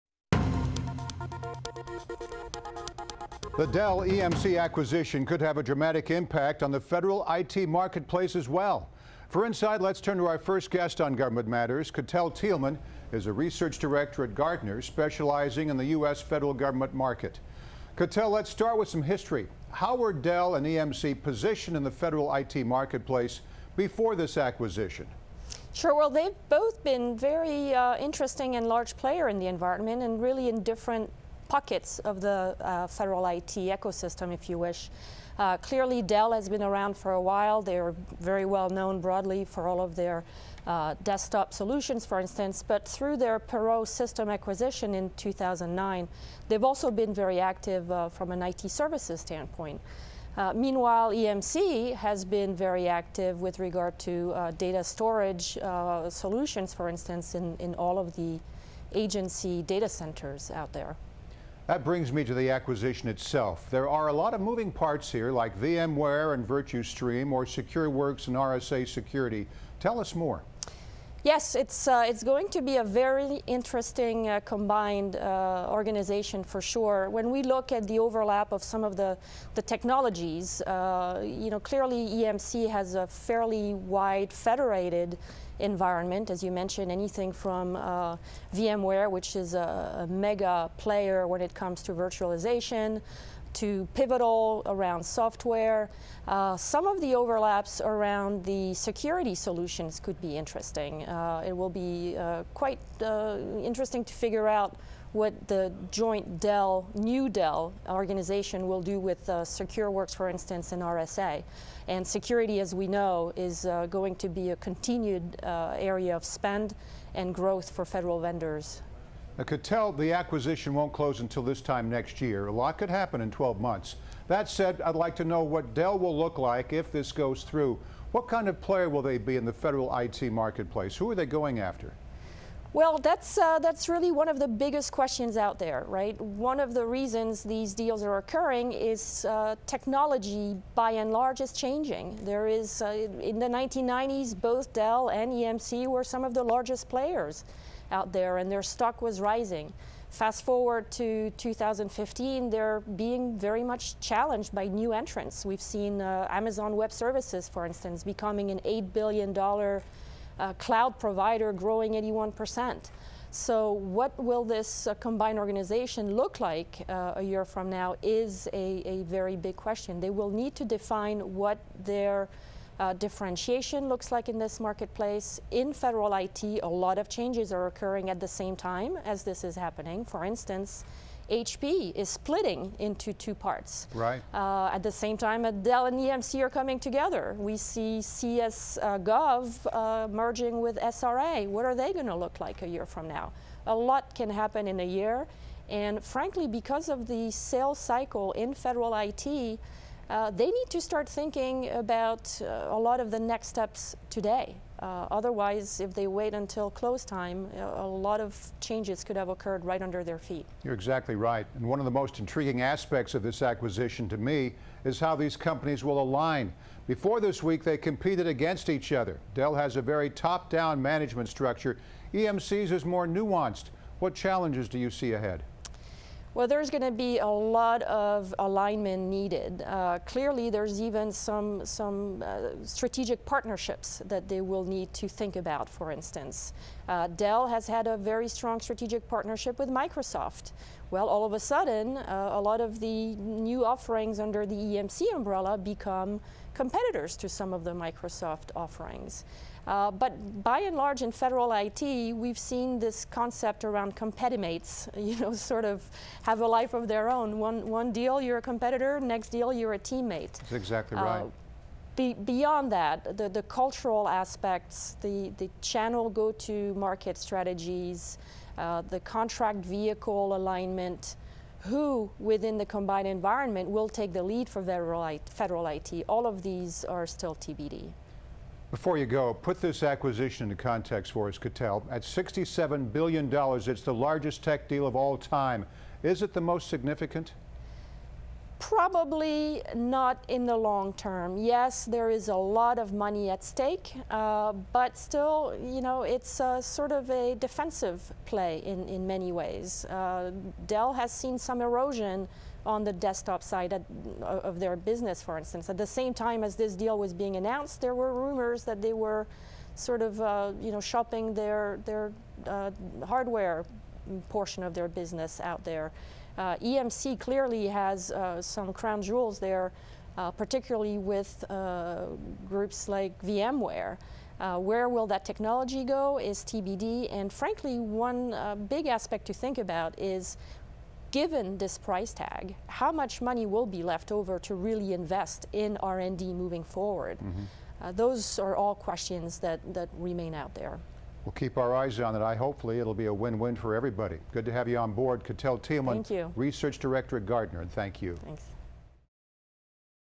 Alignment challenges ahead for Dell and EMC
In the largest technology deal of its kind, Dell announced the acquisition of data storage provider EMC for $67 billion dollars.

The personal computer company has been trying to expand its offerings by becoming an IT solutions provider as well.

Gartner research director Katell Thielemann discussed what impact the Dell acquisition could have on the federal IT marketplace.

Click the play icon below to watch her interview with Government Matters.